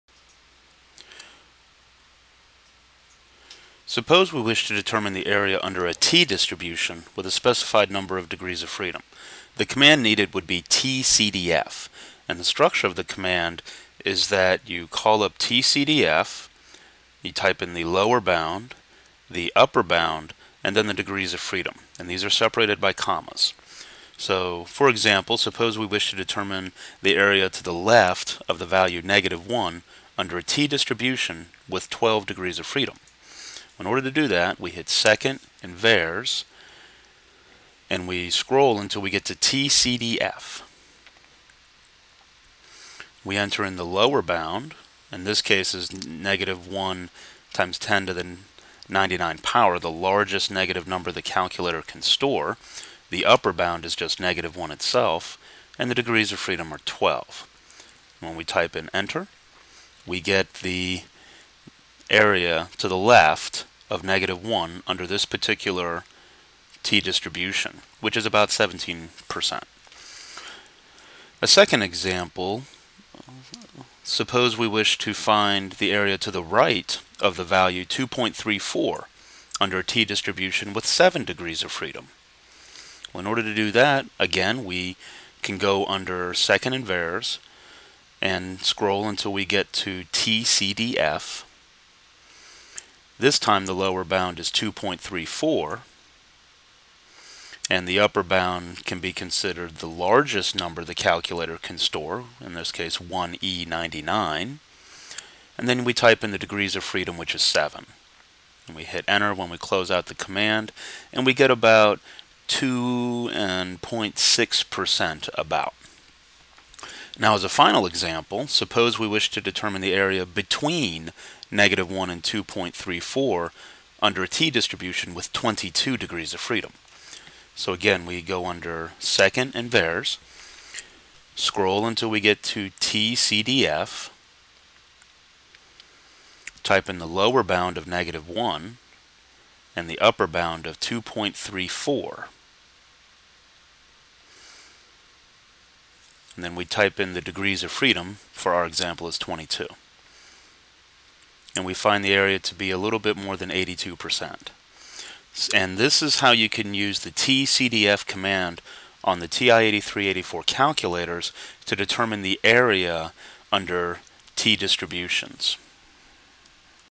tcdfcommand
This video demonstrates how to use the tcdf command on the TI-83/84 calculators.